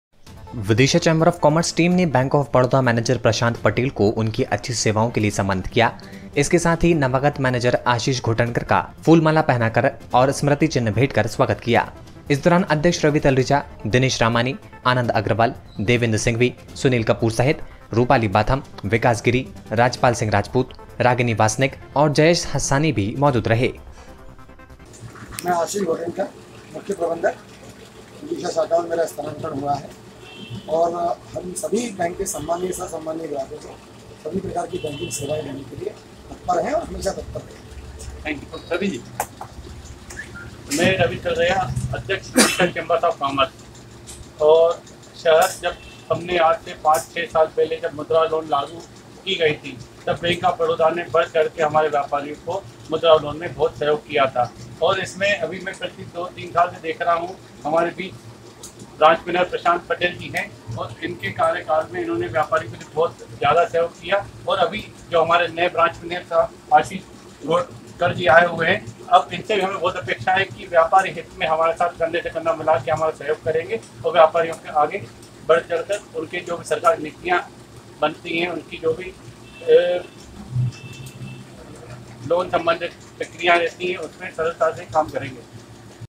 विदिशा बैंक ऑफ बड़ौदा के नवागत मैनेजर का किया स्वागत
विदिशा चेंबर ऑफ काॅमर्स टीम ने बैंक ऑफ बड़ौदा मैनेजर प्रशांत पटेल को उनकी अच्छी सेवाओं के लिए सम्मानित किया करने सहित नवागत